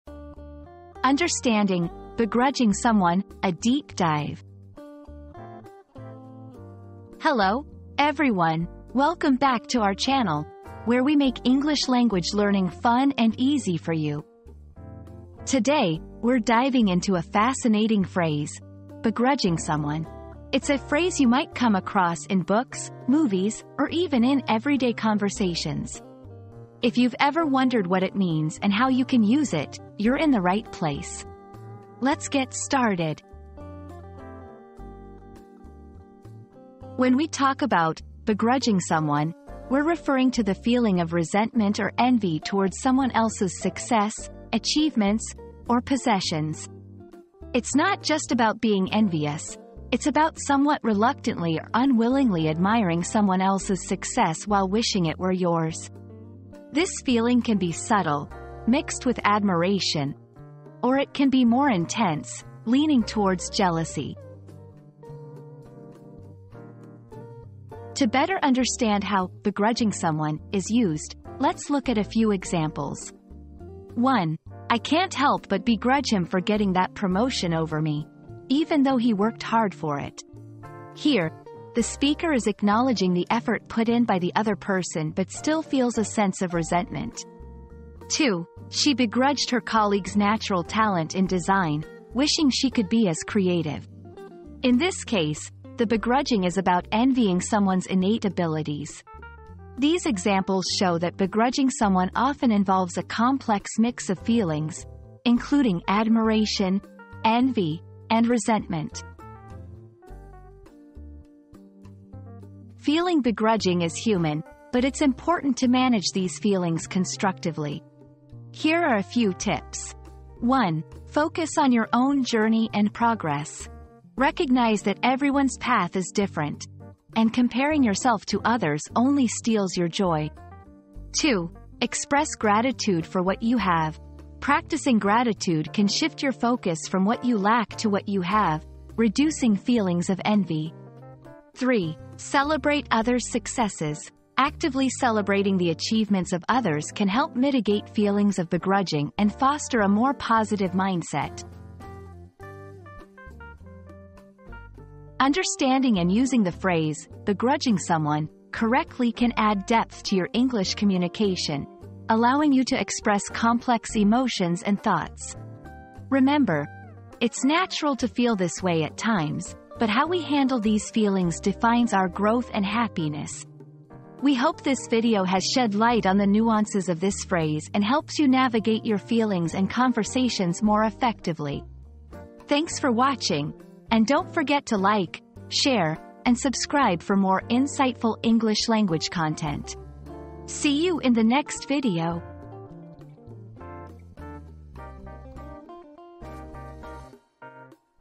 Understanding "Begrudging Someone": A Deep Dive
Understanding 'Begrudging Someone': A Deep Dive • Discover the true meaning and intricacies of 'begrudging someone' in this thought-provoking deep dive. Uncover the hidden emotions and complexities behind this common human experience and gain a deeper understanding of empathy and forgiveness.

00:00 • Introduction - Understanding "Begrudging Someone": A Deep Dive
00:36 • What Does "Begrudging Someone" Mean?
01:07 • Examples of "Begrudging Someone" in Everyday Use
01:52 • How to Overcome Feeling Begrudging